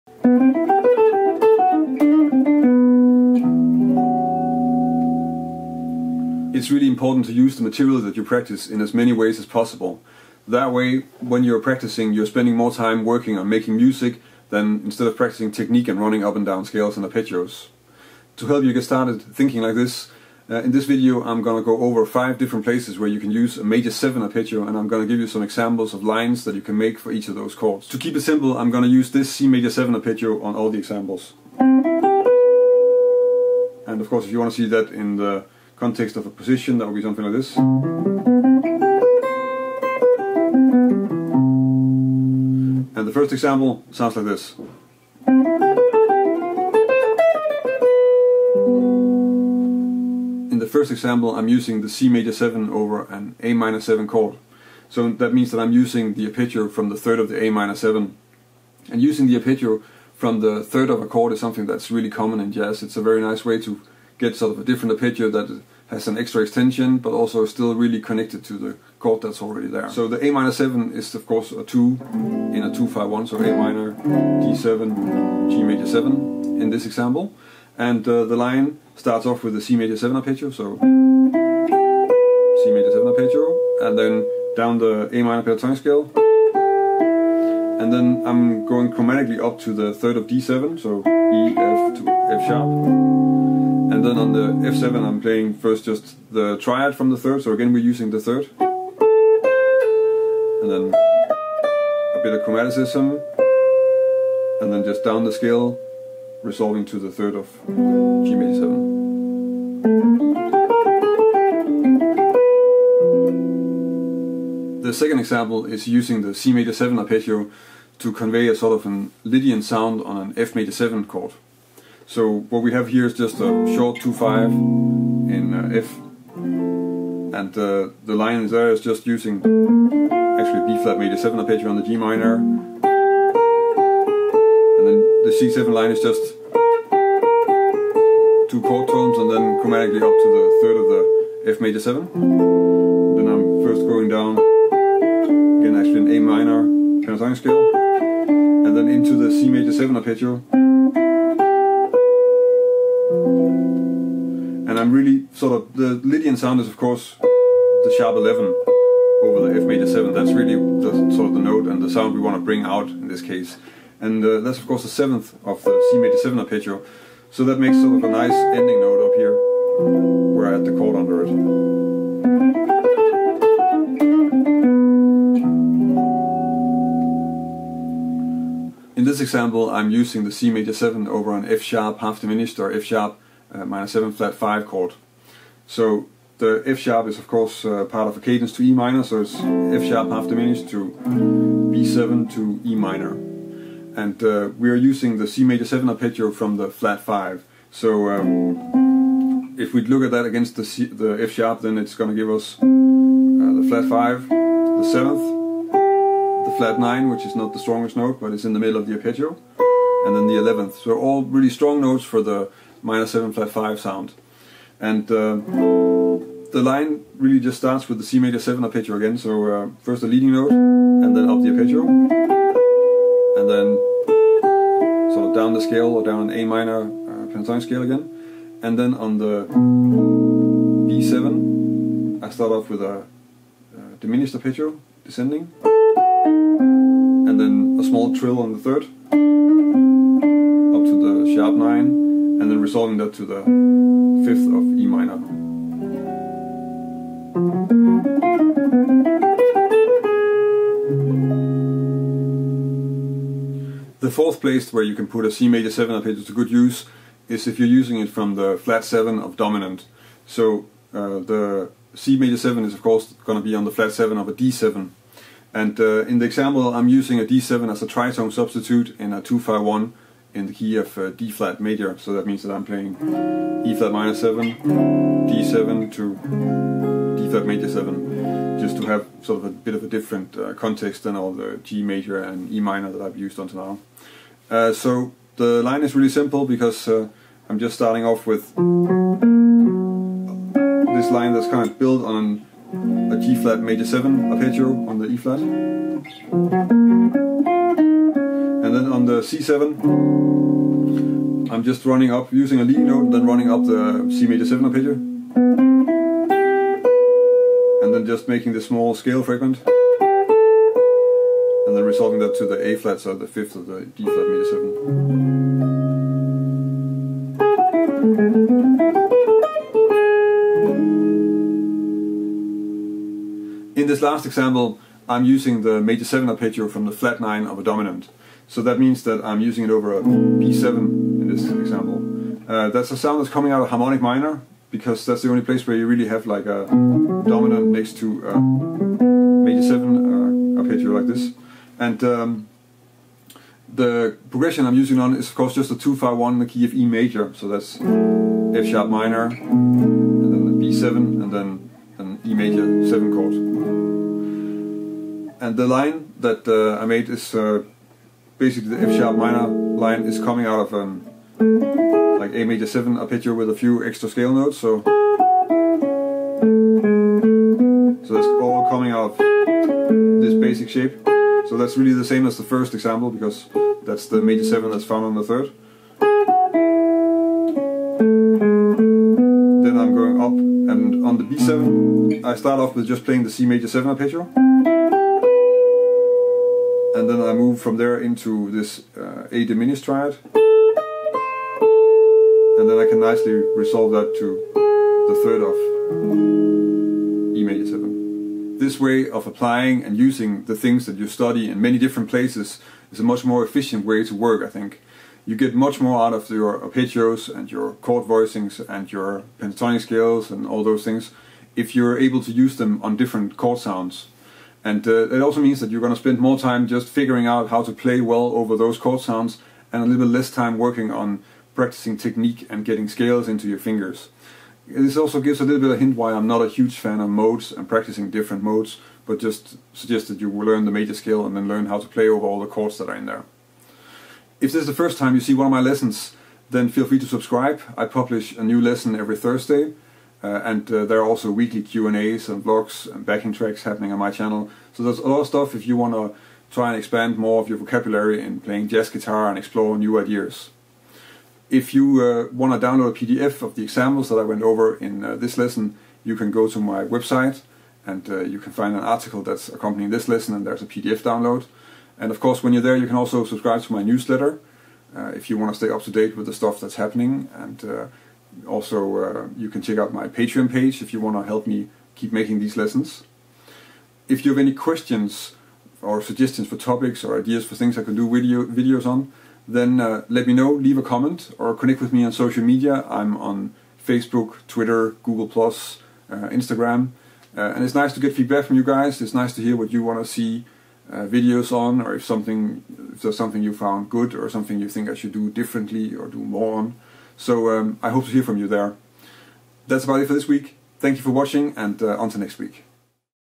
Are you using your Maj7 arpeggios on these chords ❓ - Superimposing Arpeggio - Jazz Guitar Lesson
It is very important to use the material that you practice in as many ways as possible! That way you spend more of your practice time making music than working on technical exercises with scales and arpeggios.

In this jazz guitar lesson I am going to show you how you can use the same Maj7 arpeggio and superimpose it over 5 different chords and give you some examples of how that might sound in a jazz line!

You can download a PDF and read the article on my blog via this

My Gear:
Thomastik Bebop 13 Roundwound Strings
QSC K10 Powered Monitor
FocusRite Audio Interface
Sennheiser HD215 Headphones
Lumix G6 Camera
GuitarPro 6